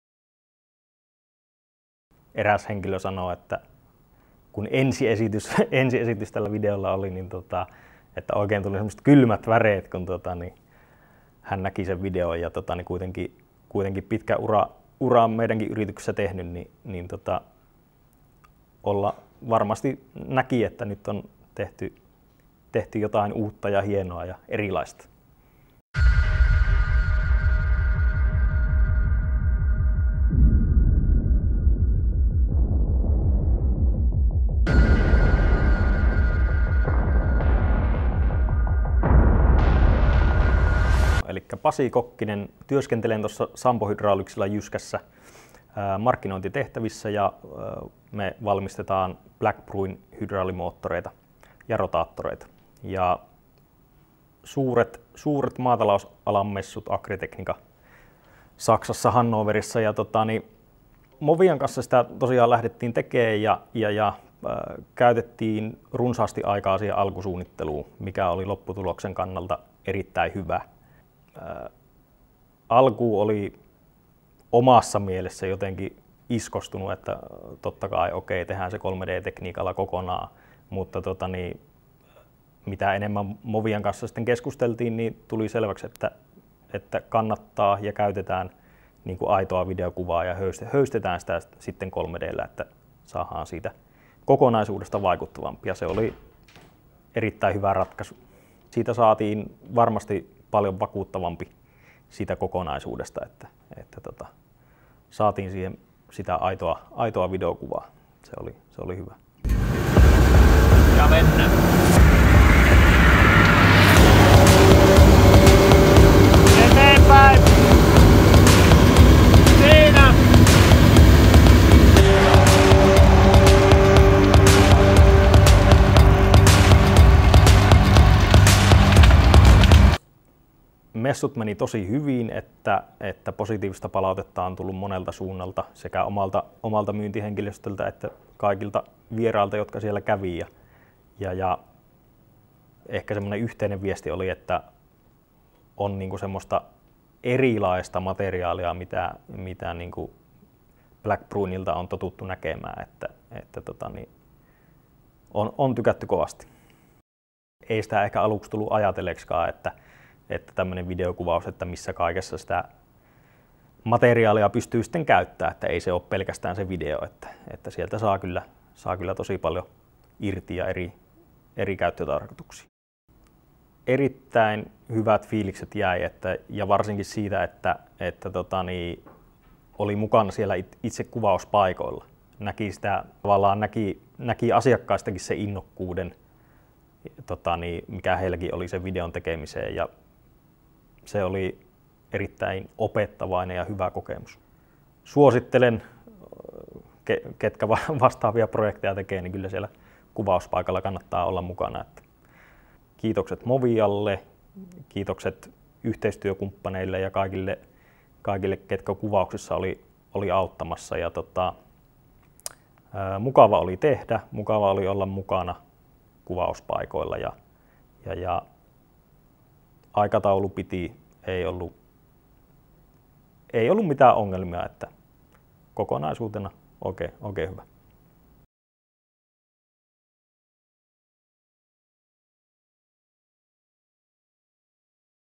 Behind the Scenes of Black Bruin On-Demand Wheel Drives
Interview with Pasi Kokkinen from Sampo-Hydraulics and a look behind the production of Black Bruin On-Demand Wheel Drives promotional.